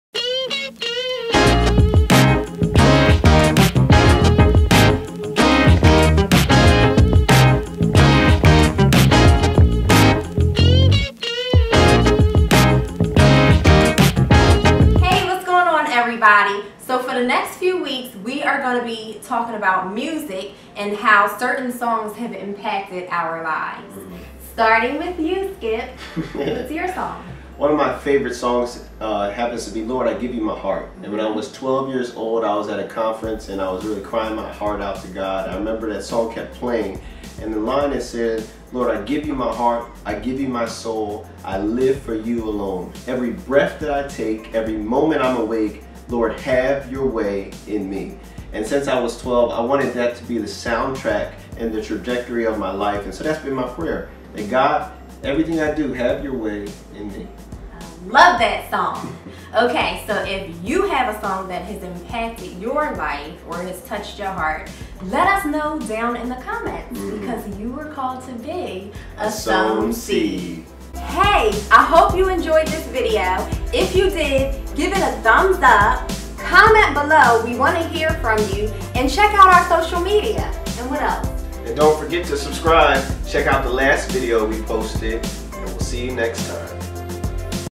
The Soundtrack of My Life
Music can play an influential role in our lives. When you think of the songs and lyrics that have impacted you the most, what's the soundtrack of your life sound like? Check out this video to hear about one song that helped shape my life.